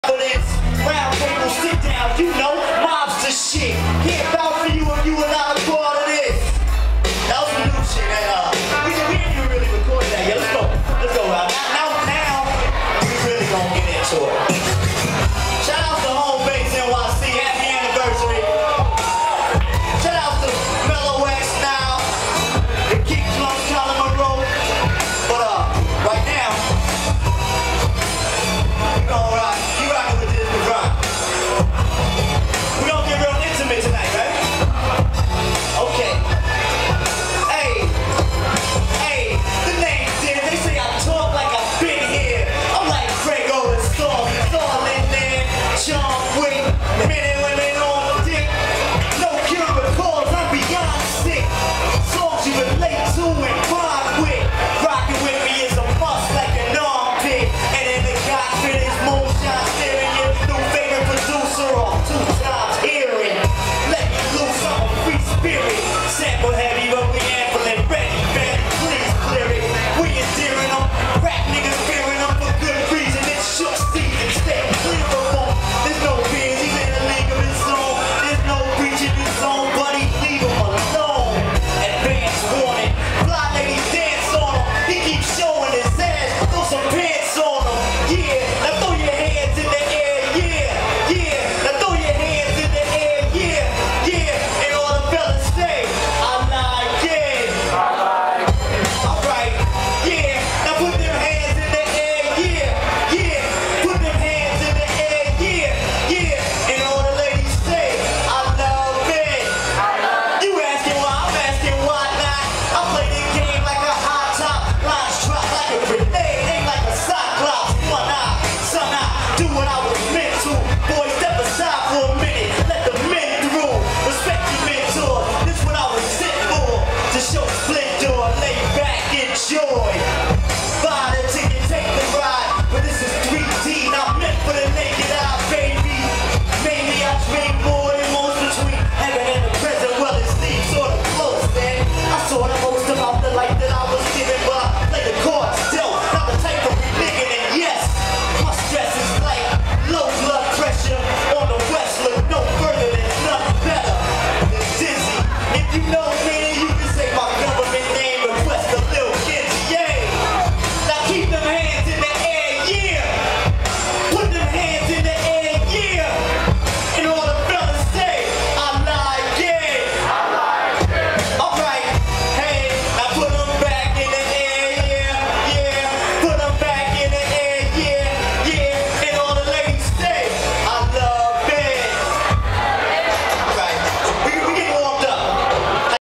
Diz Griban -
performing his track during the Hombase 1 year anniversary at SOB's in New York City